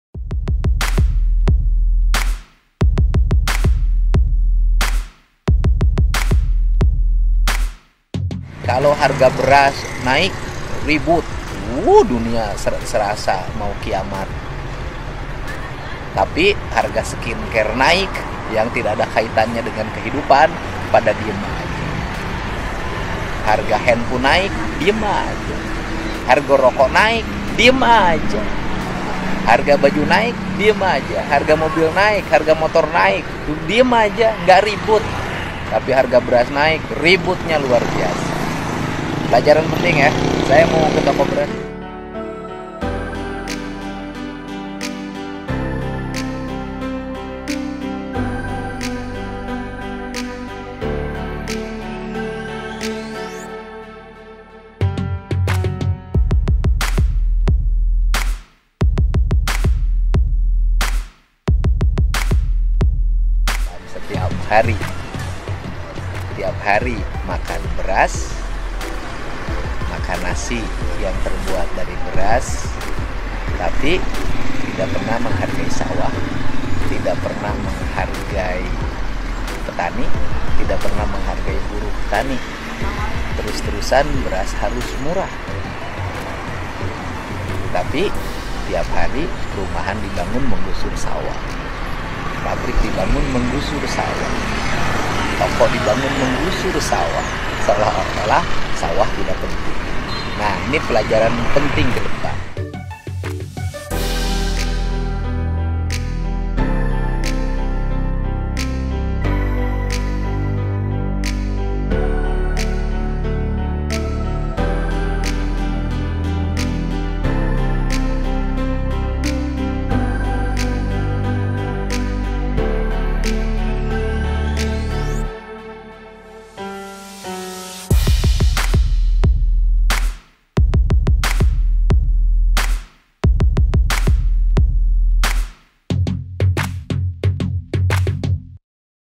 Dedi Mulyadi: "Skincare-Rokok Naik Tetap Dibeli, Beras Naik Serasa Mau Kiamat"
VIVA - Dedi Mulyadi berkomentar terkait reaksi masyarakat yang mengeluh dengan kenaikan harga beras di sejumlah wilayah.Dedi Mulyadi heran semua orang berteriak saat harga beras naik seolah dunia mau kiamat.

Namun, ketika barang yang tidak ada kaitannya dengan kehidupan juga mengalami kenaikan harga, semua diam.“Kalau harga beras naik ribut, wuuhh dunia serasa mau kiamat,” ujarnya di YouTube Dedi Mulyadi Channel.

[R-DA]
Reporter : -
Kontributor : -